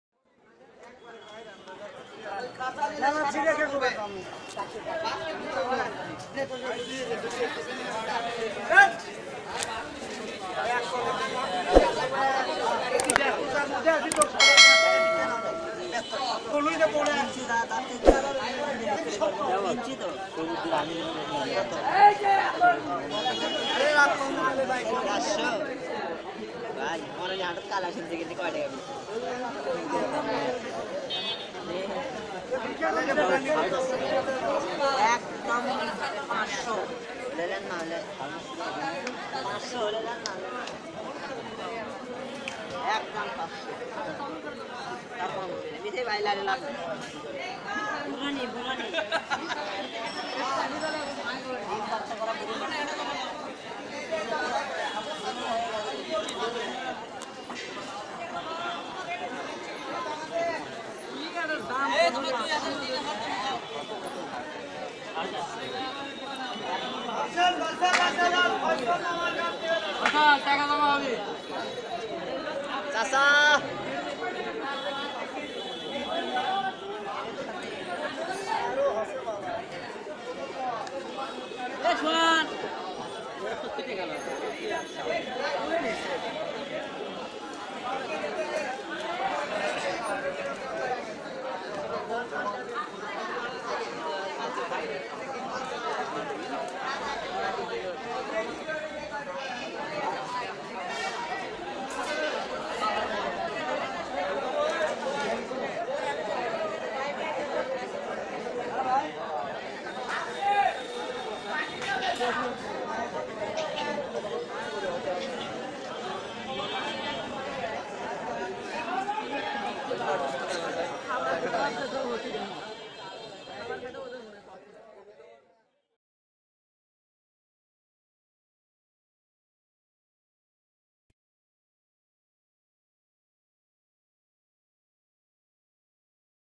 ►Various Pigeon Selling at Local Market II Buy-Sell Birds HAAT II Open Market-Real Life Activities
►Various Pigeon Selling at Local Market II Buy-Sell Birds HAAT II Street Seller BD II Open Market Seller BD II Real Life Activities in Present BD

Beloved Viewers…There is No Fake Scene shown here. Everything in This Content is Real. So, I Can Proudly Say That You Will Get 100% Original Flavor and a Lot of Experience With Bangladesh.